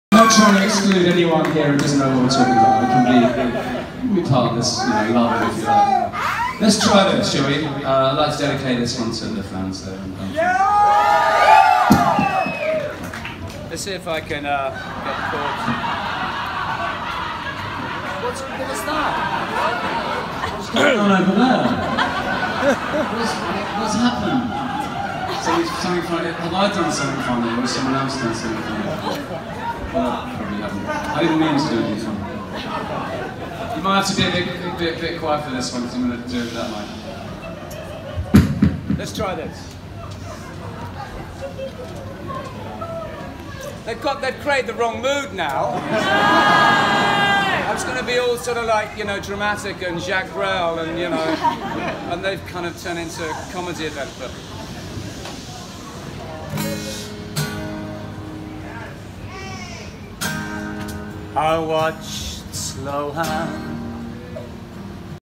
Brett Anderson talking, Bristol
Brett Anderson introducing his acoustic slot, Bristol O2 Academy, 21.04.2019.